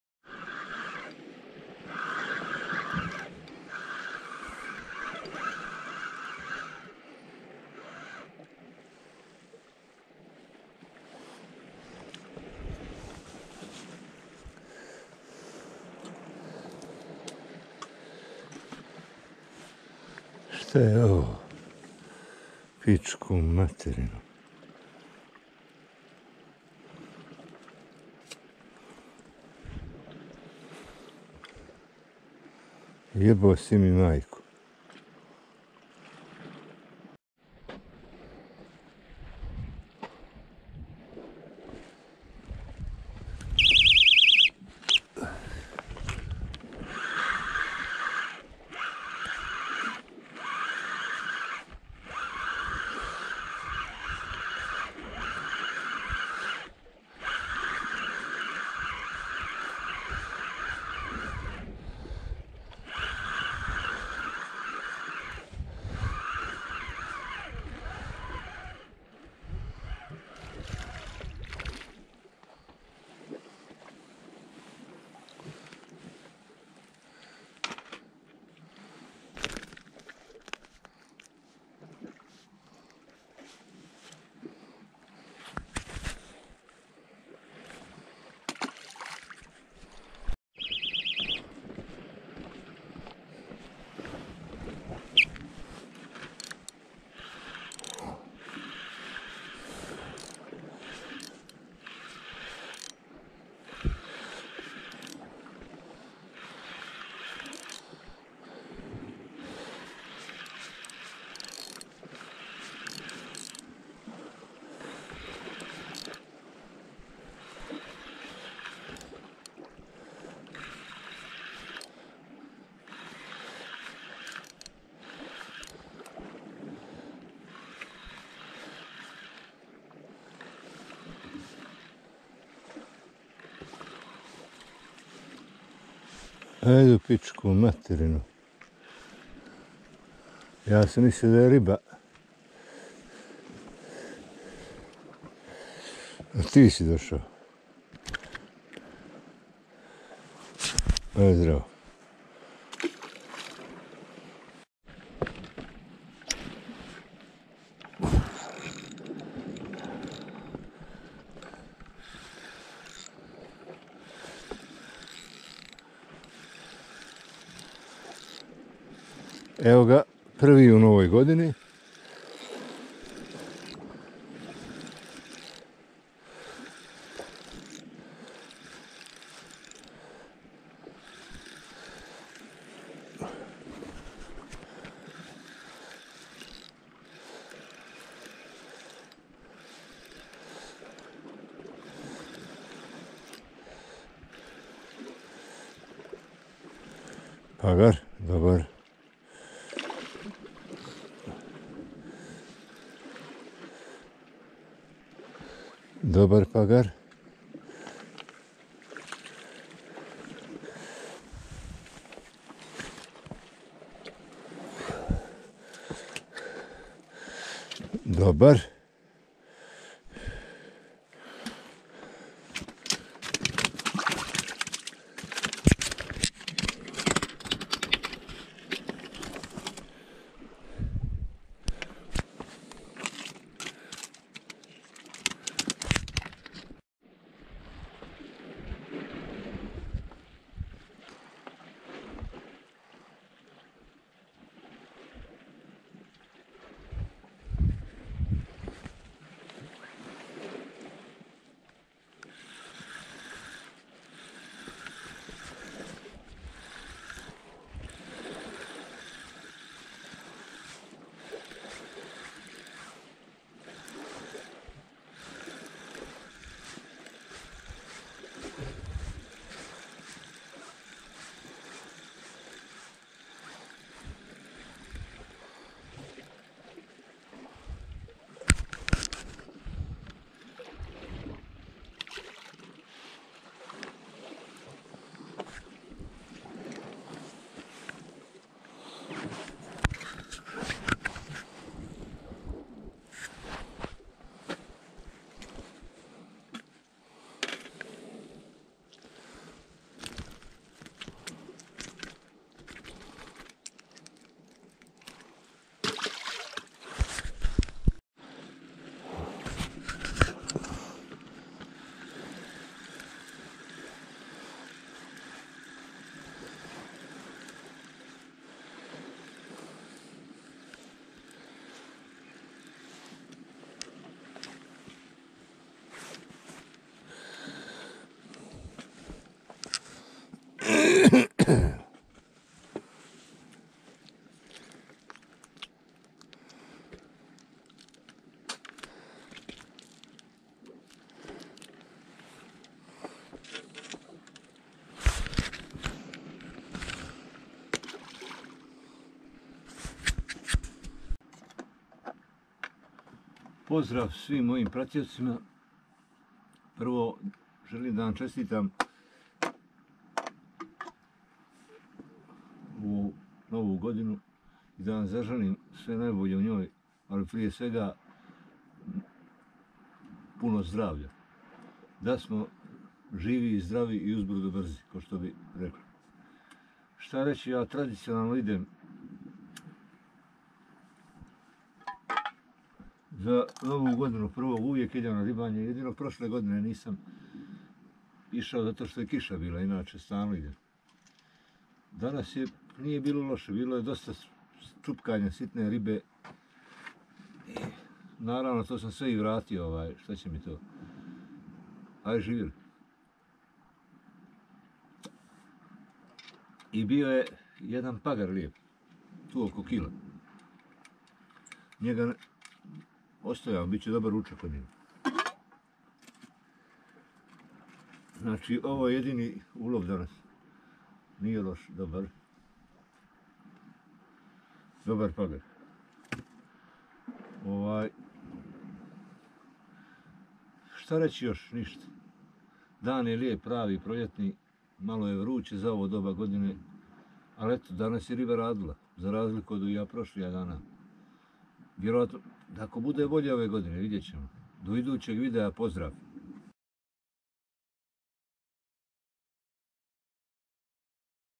Ribolov štapom s obale-Prvi ovogodišnji ulov(Pagar oko 1 kg.)
Srećna i berićetna Nova 2023 god i da nas zdravlje služi da budemo uz brda brži.Šta reći o početku ove god nije bilo tako loše riba je radila do 10 sati i ako je bio samo ovaj jedan Pagar da valja stalno je bilo akcije tako da mi je vrijeme brzo proteklo.Što se tiče mamca danas je bolje radilo na Lignju , Srdelu je sitna riba raskubala prije nego potone udica ostane gola.Sve kad se sabere nije loše za početak ove god samo da ovako i potraje.